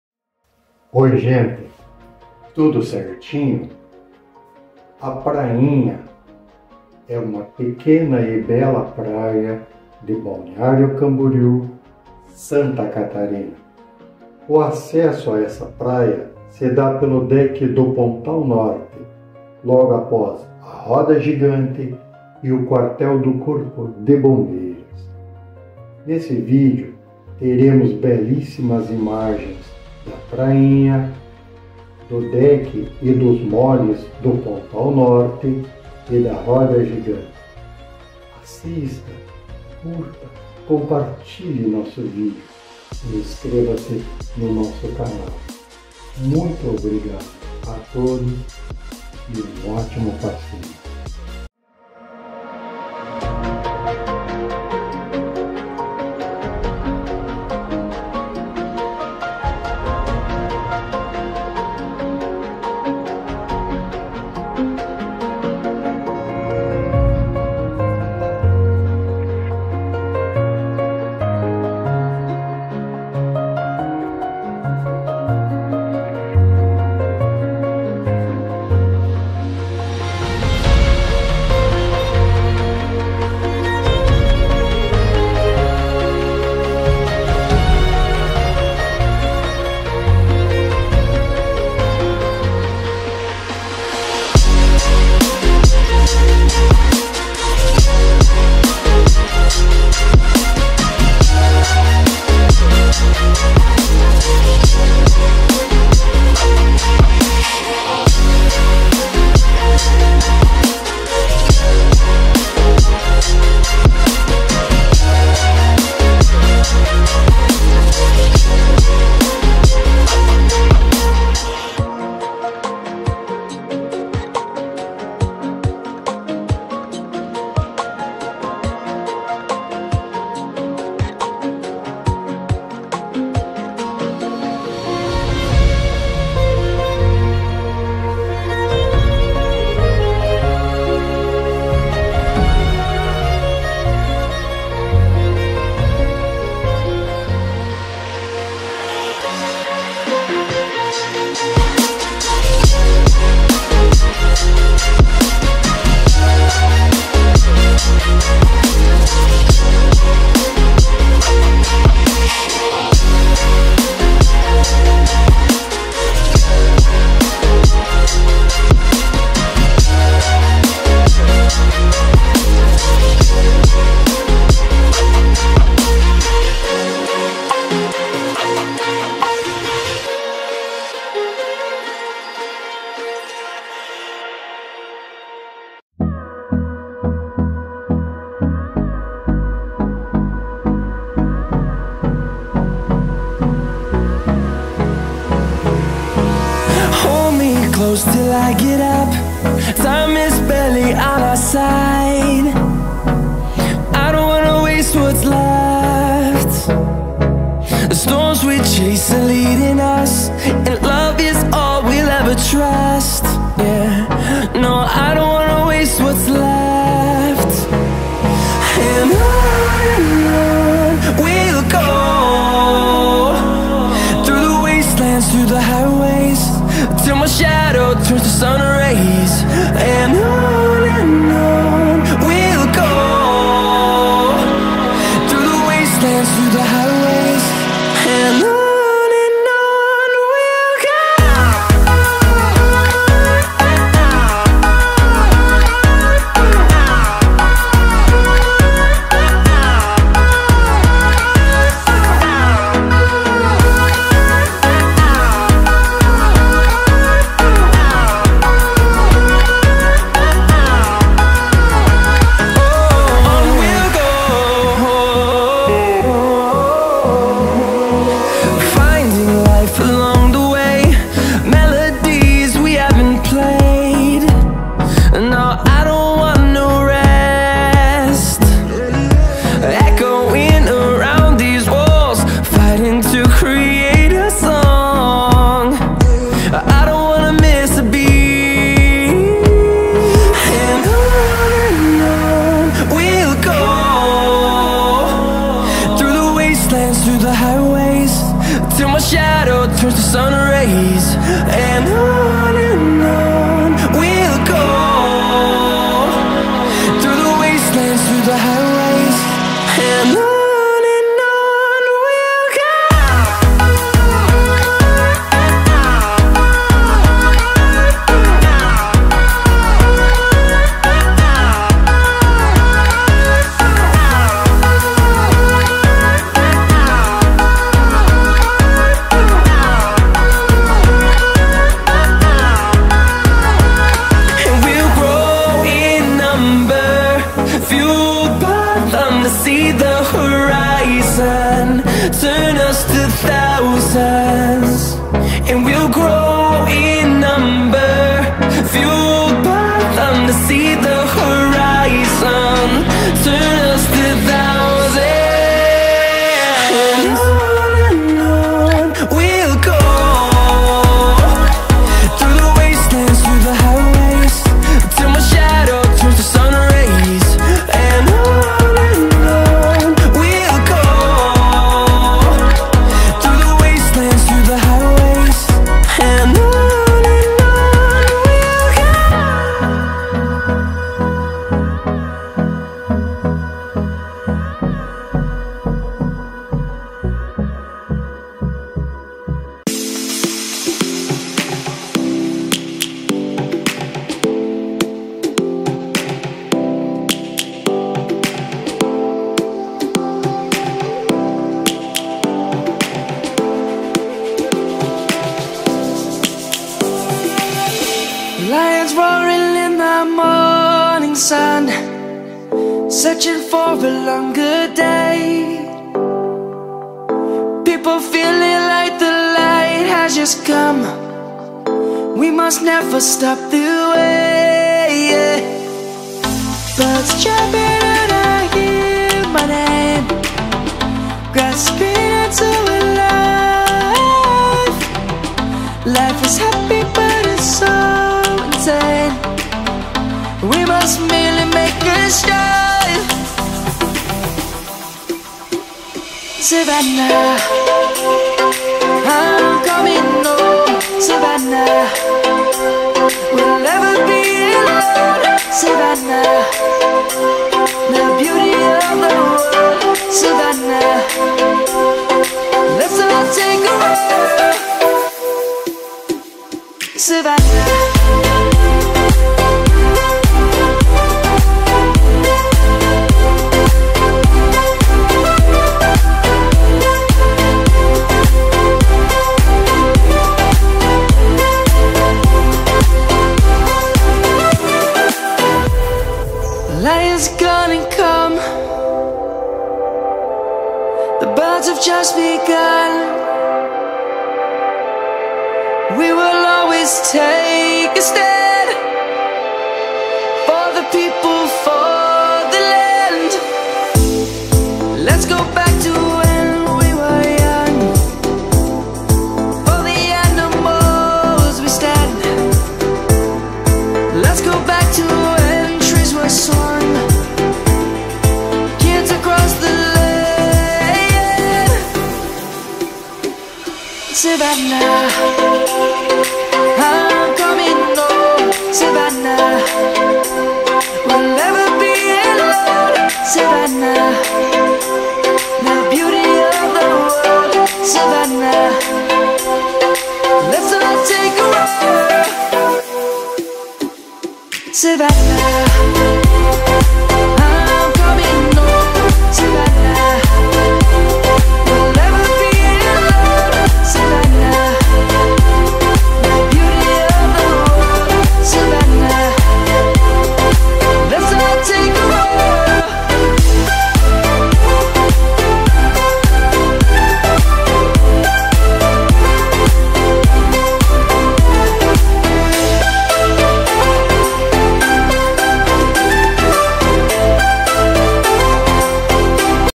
Voo de drone na Prainha em Balneário Camboriú - Santa Catarina
Voo de drone na Prainha, que é uma praia próxima ao Pontal Norte, em Balneário Camboriú - Santa Catarina.
Nesse vídeo faremos um voo de drone pela linda Prainha, em Balneário Camboriú, Santa Catarina.
Durante o vídeo veremos imagens da Prainha, da Praia Central, Molhes do Pontal Norte, Deck do Pontal Norte e Big Weel, a roda gigante.
00:00 - Introdução do vídeo.
00:19 - Molhes do Pontal Norte.
00:50 - Prainha guimbal baixado.
01:37 - Big Weel - roda gigante de Balneário Camboriú.
02:34 - Giro panorâmico com vista da Praia Central. e Ilha das Cabras.
03:21 - Vista da Prainha, Deck do Pontal Norte, Molhes do Pontal Norte e Big Weel.
03:50 Vista da Prainha com câmara do drone abaixada.
04:28 - Vista do Deck do Pontal Norte e Prainha com o drone em voo baixo.
04:56 - Vista da Praia Central e Molhes do Pontal Norte com o drone em voo baixo.
05:38 - Prainha vista mais de perto.
07:16 - Prainha vista com a câmara do drone abaixada.
09:05 - Pousando o drone.